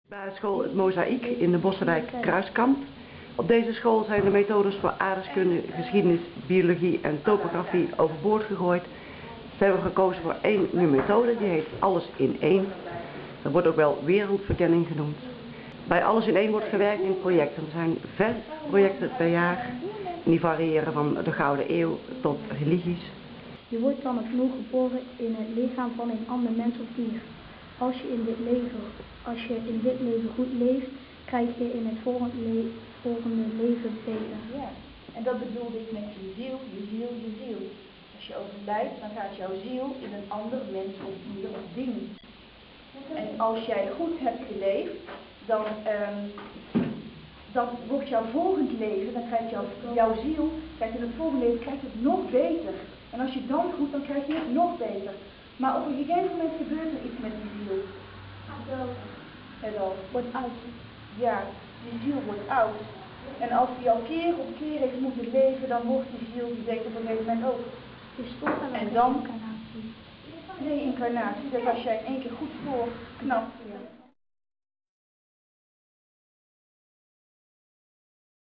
Wereldverkenning op 't Mozaiek
Den Bosch - Aardrijkskunde, geschiedenis en biologie zijn geen aparte vakken meer op basisschool 't Mozaiek in de Bossche wijk Kruiskamp. Zie ook